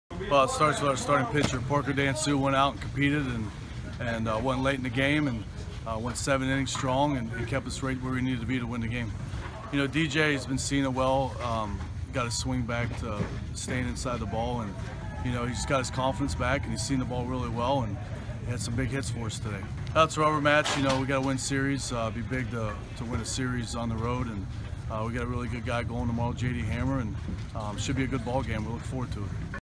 Head Coach Jeff Waggoner Postgame Video at FIU
Head Coach Jeff Waggoner after winning the second game of the three-game series at FIU.